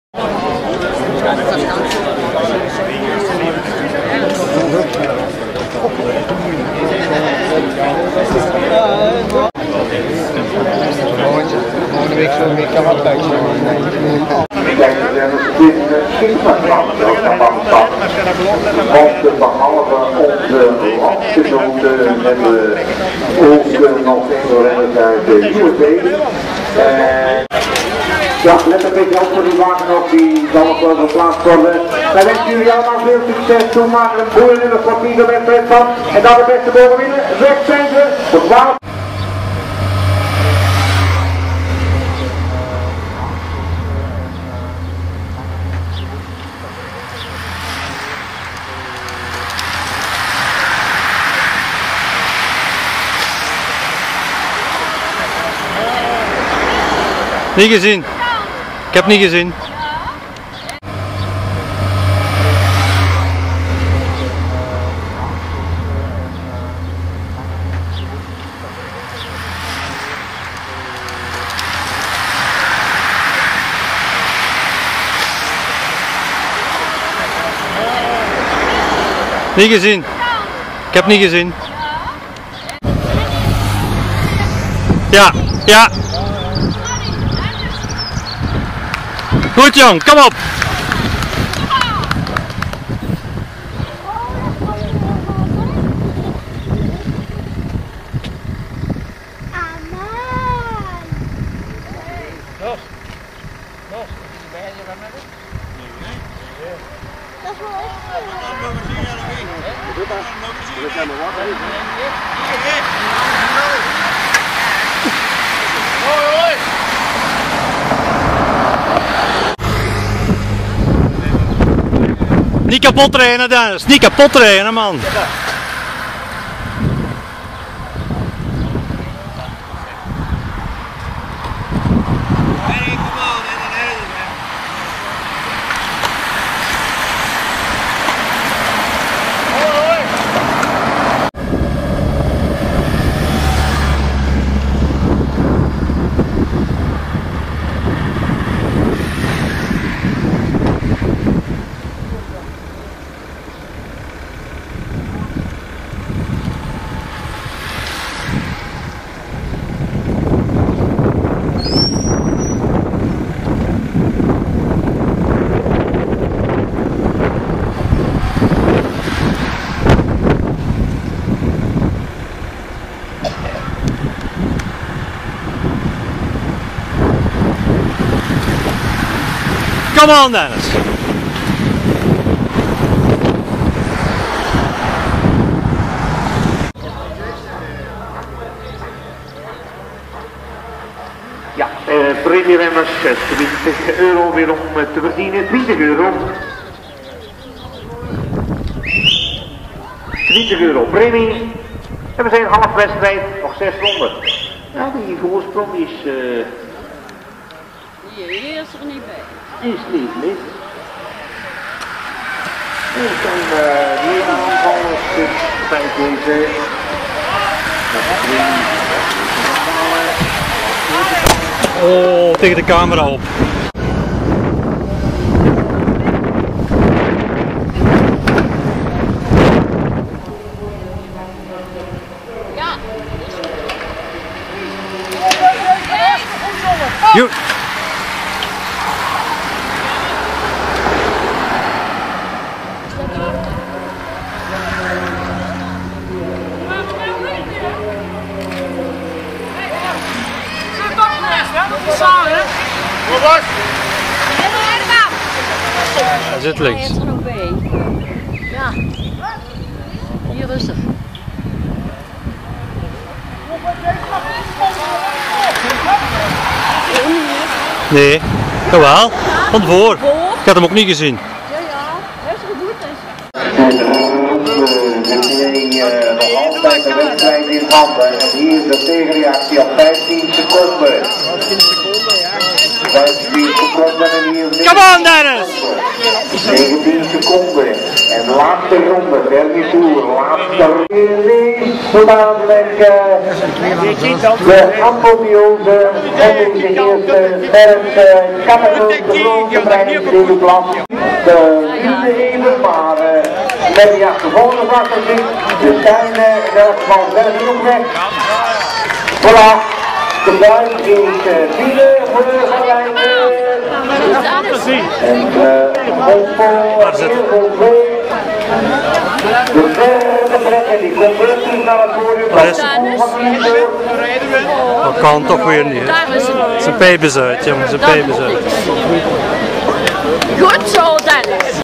Koers junioren Meerhout
koers junioren meerhout 23/06/12
12de van de 60 deelnemers
100 km koers ( 7,5 km/ronde)